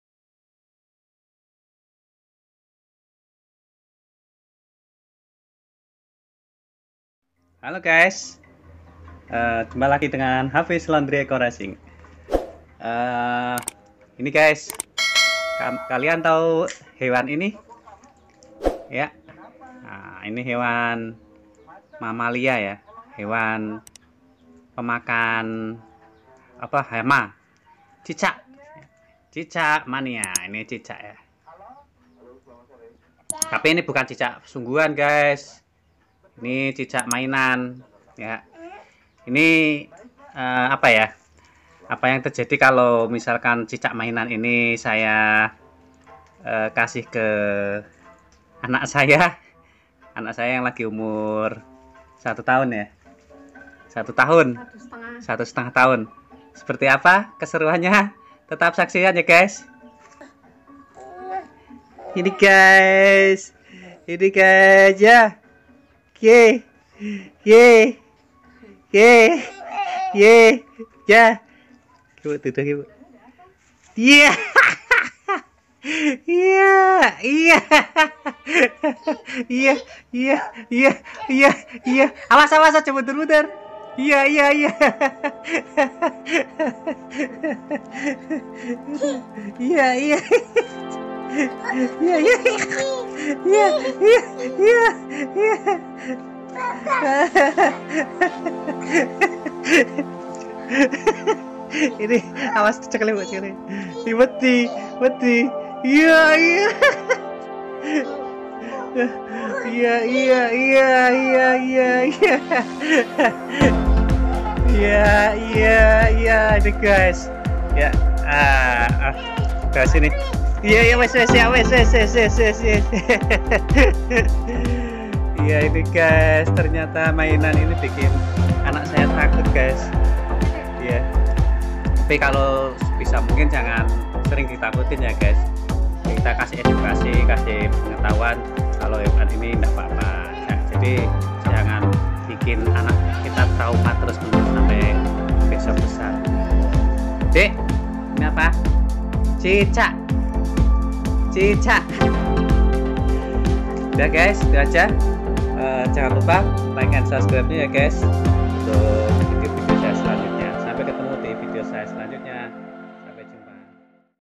Cicak mainan vs anak kecil
Lucu banget reaksi anak kecil umur 1 thun guyz, di lihatin cicak mainan dari karet takut bangt si dedek nya. Untuk edukasi kita aja klo anak kecil jangn sering di takut"i dengan hal"kecil sekalipun kita orang tua mengedukasi ke ank klo mainan jangn di takuti dan sebagainya. Ok sampe jumpa di video saya selanjutnya ya?

cicakmainanvs# anakkecil# satutahun